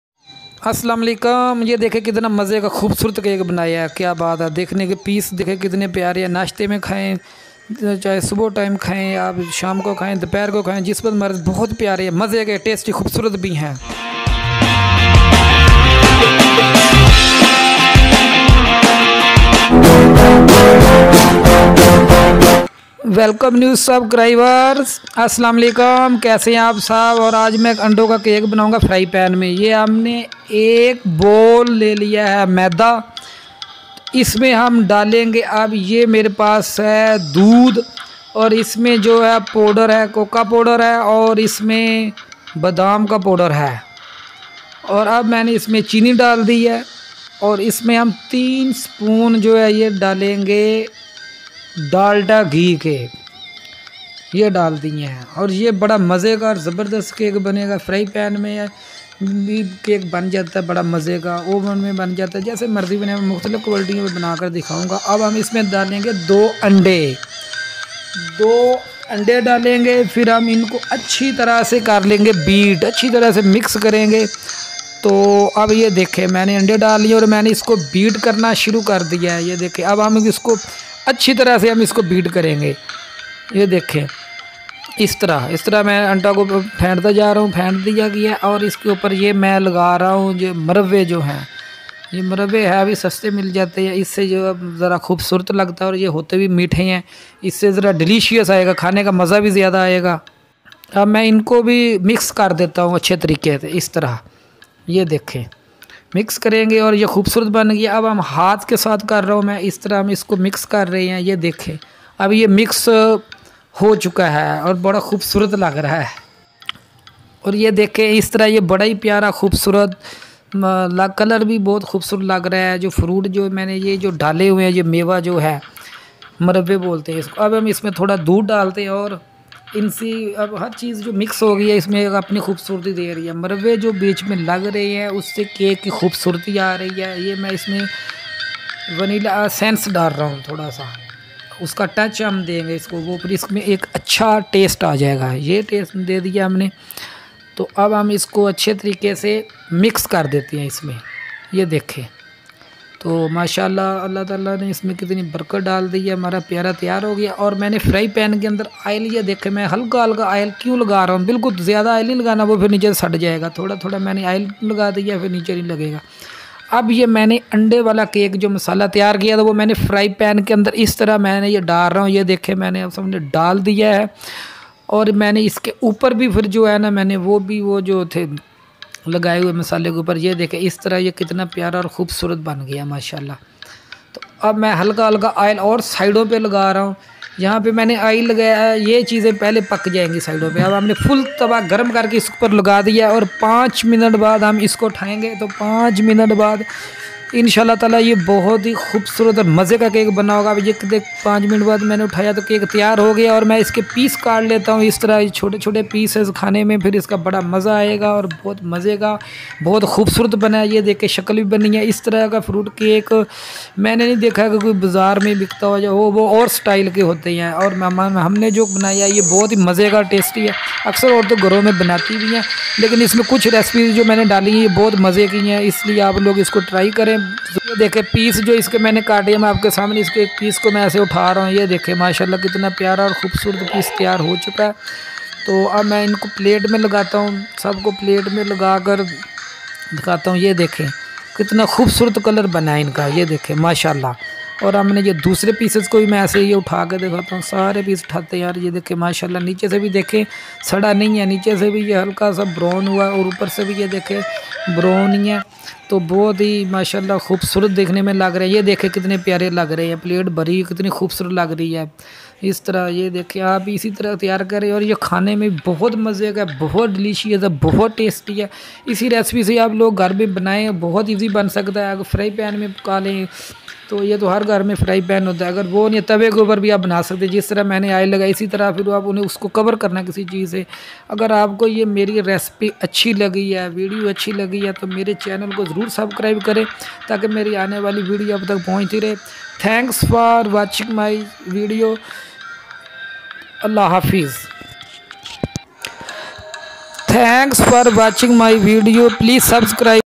5 Mint Cook | Fruit Cake Recipe | Homemade Fruit Cake | @Rana Chef International
5 Mint Cook | Fruit Cake Recipe  | Homemade Cake @RanaChefInternational_786  :-

Ingridents:
 1.  1 cup fine floor
2.  1 tea spoon coconut powder
3. 1/2 cup marba
4. 1 tea spoon almond powder
5. 2 table  spoon milk
6. 3 table spoon  dalda ghee
7. 2 eggs
8. 1/2 tea spoon  vanila essence

fry paean mein  1/2  tea spoon oil  lagin aura cake dail dain heat halki kr dain aura tawa gram kr k 5 mint k lia fry pane k opr rekh dain maza ka cake bna ga

For more more videos  related  cooking

Contct Yoytube .

Face book.